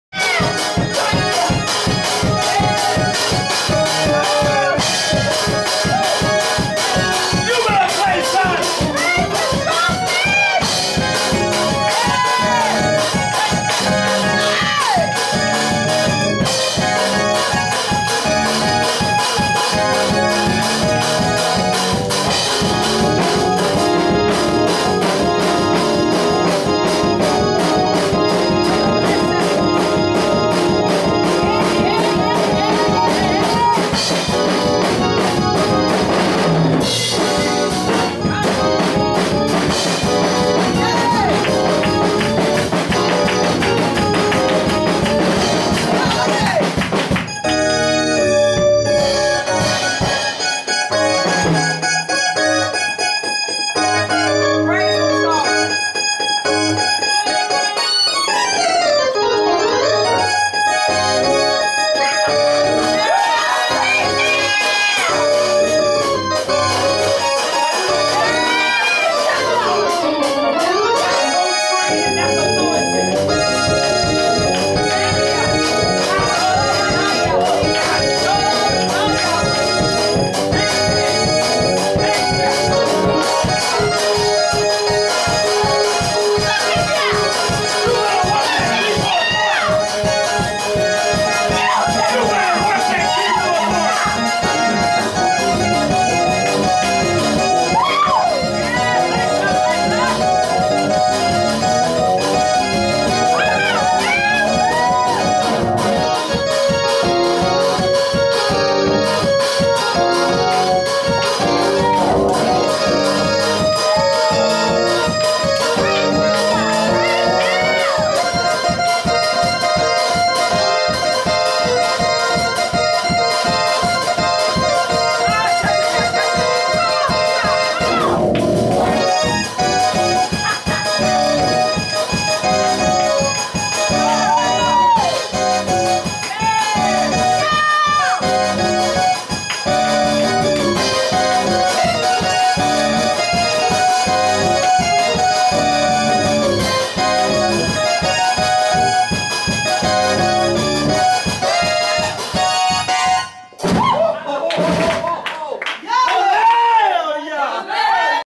Love of Life Assembly Yahweh's Temple of Praise
The Apostle's son, Charleston Shemuel Yancey shows his spirit filled skills on the keyboard. Charleston has had no music lesson and only been playing keyboard for three months. He left playing the drums for which he was also anointed to play and ask his father could he play the keyboard. We are proud of what Yahweh is doing through him.